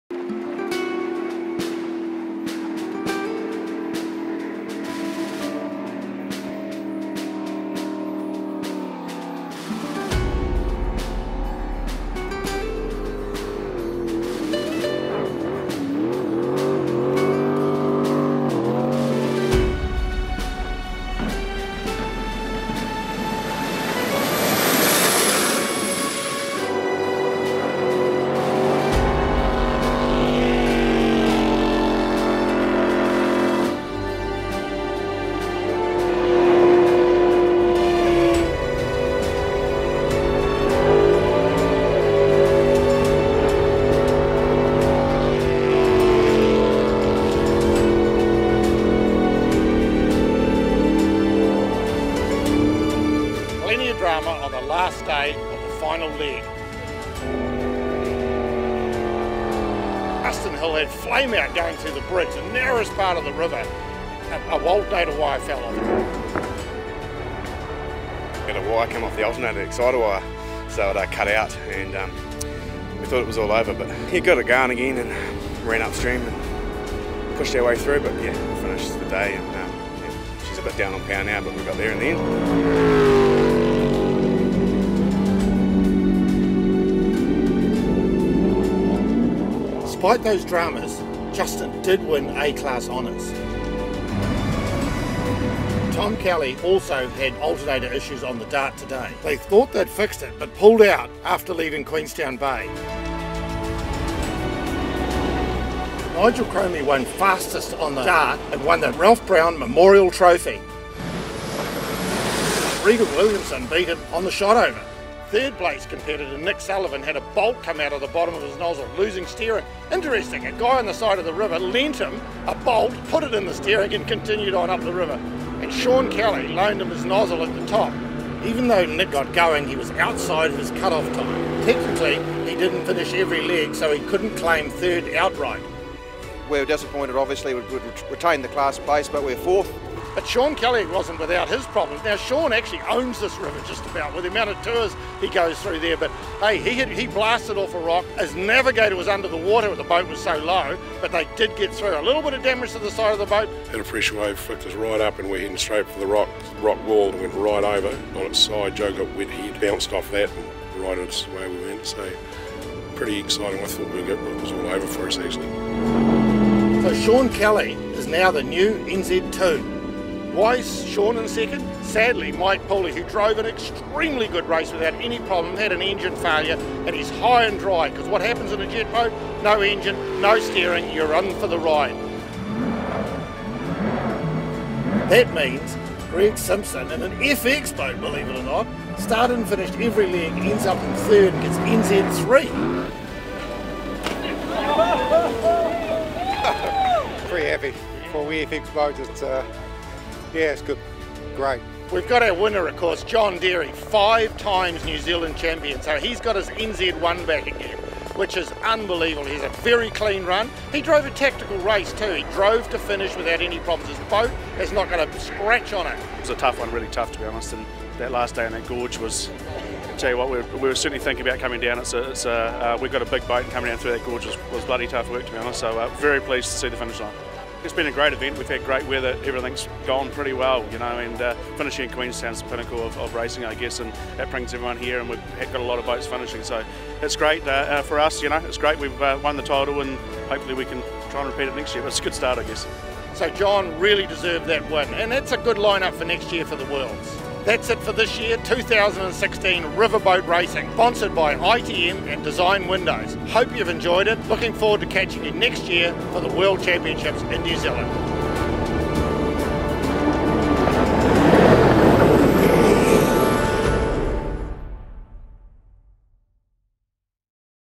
19 Day 7 - Kawarau / Shotover Rivers - Final Wrap
Well here it is... the final instalment in this year's ITM Design Windows New Zealand Jet Boat Marathon TV coverage. Right to the end nothing could be taken for granted and the results are a testament to a consistent managed approach to a week of river racing. Congratulations to everyone on a fantastic marathon. Thanks to our major sponsors ITM and Design Windows and a special thanks to media partners K-Jet Queenstown and Mark Cromie Holden.
We hope you've enjoyed the coverage and look forward to being back again next year for the worlds.
The full playlist of coverage will be available shortly on the NZJBRRA website.
Thanks for your enthusiasm in front of the camera Mark Cromie! Cheers... from the crew at Black Magic Media